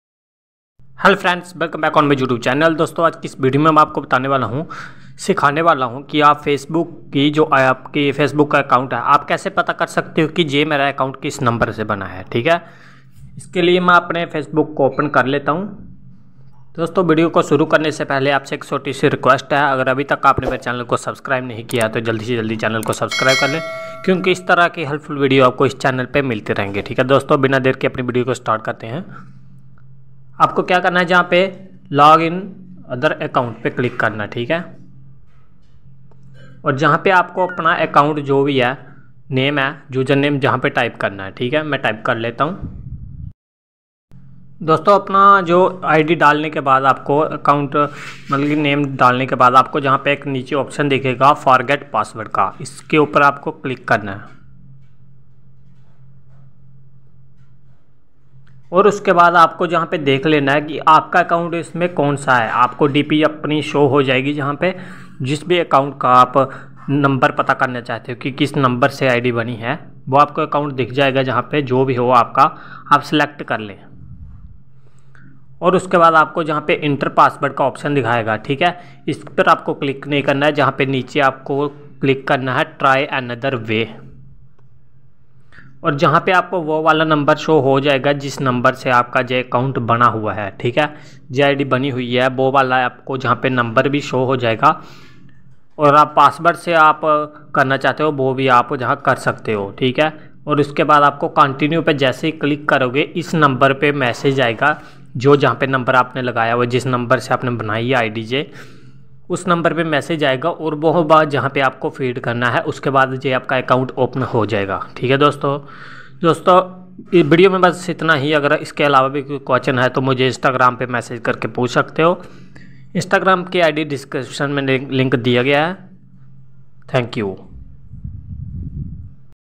Facebook id kis number se bani hai kaise pata kare||How find from which number Facebook id is made
Hello friends,
Is video me mai apko btane wala  hoon ki aap kaise pata kar saktey hai ki apki facebook ki id kis number se bani hai .

Facebook id kis number se bani hai